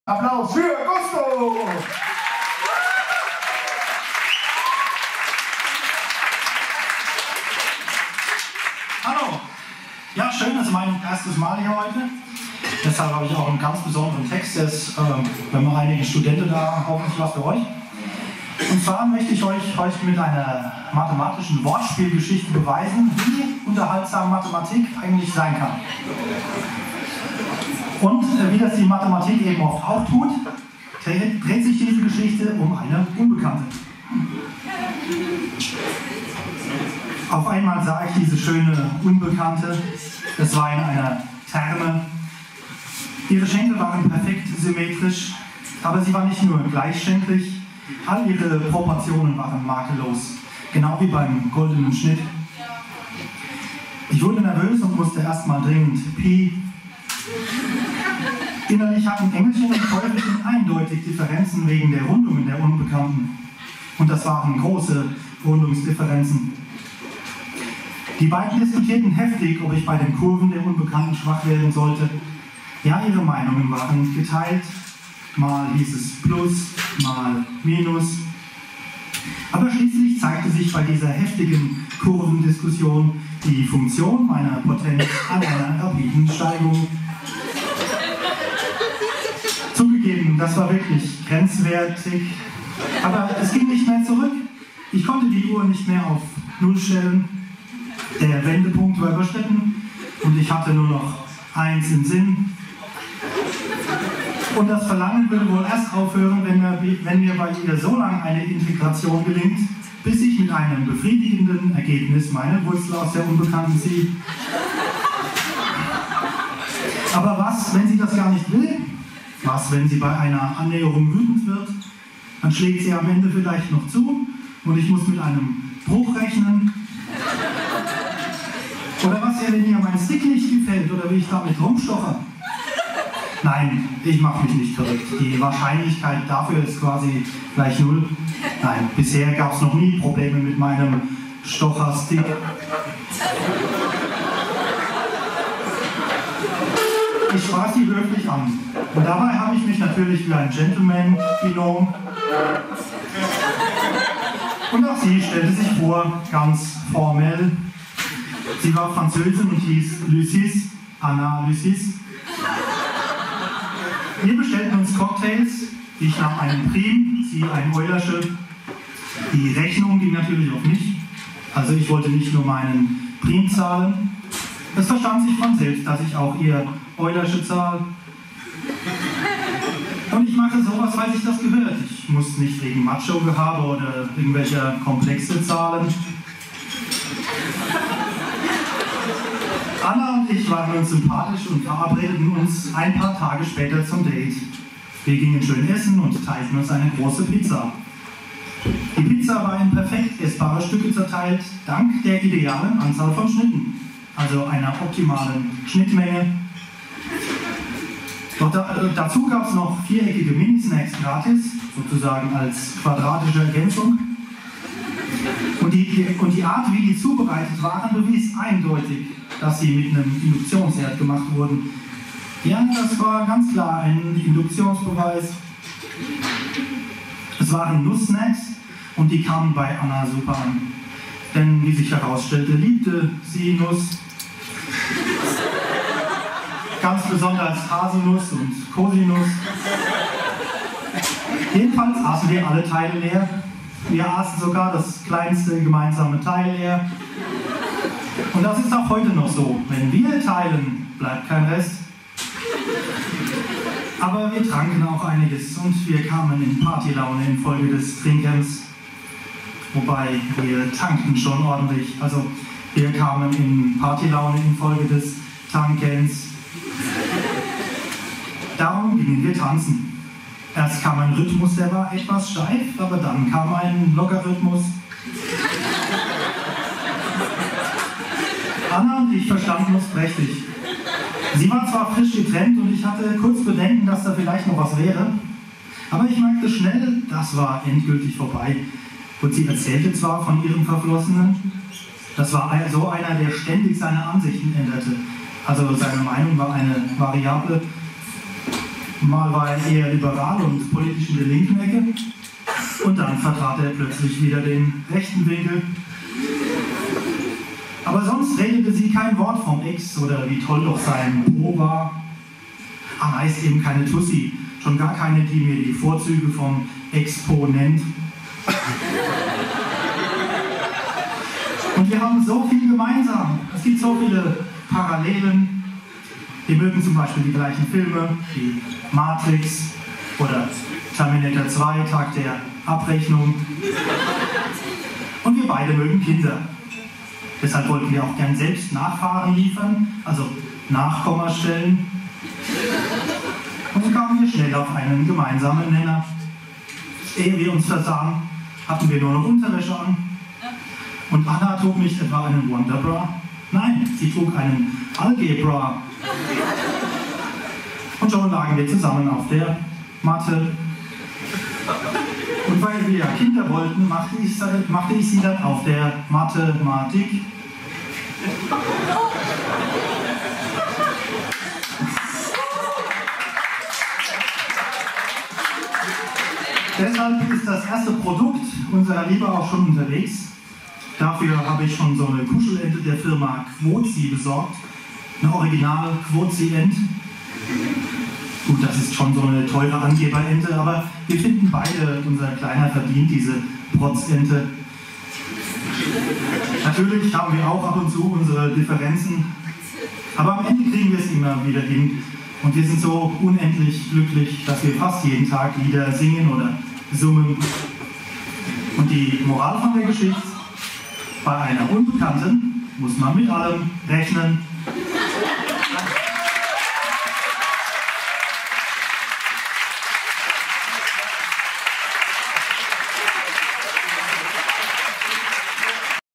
Gusto – Kurvendiskussion (Mathematik Wortspiele) Poetry Slam Mainz
Mathe mal anders:
Gusto mit einer ganz besonderen "Kurvendiskussion" beim Poetry Slam im Q-Kaff in Mainz (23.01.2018)